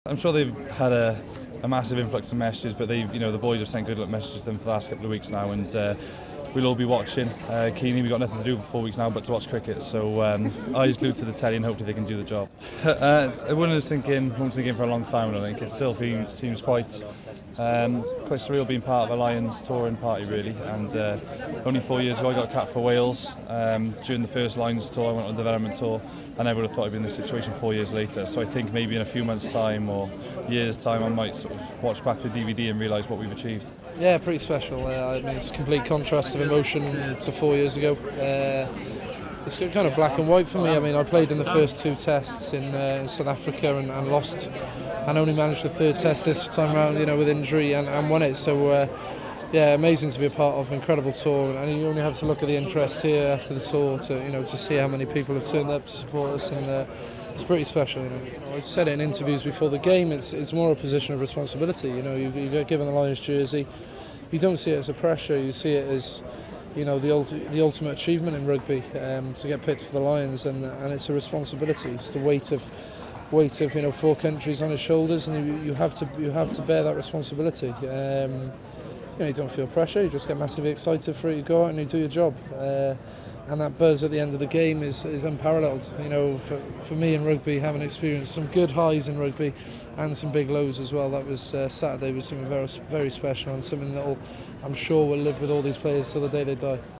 Warburton wants England to replicate Lions with Ashes win
British and Irish Lions arrived back at Heathrow on Wednesday (10th July) following their series win in Australia and victorious captain Sam Warburton had a message to send to the England cricket team, whilst Jamie Peacock reflected on a series win.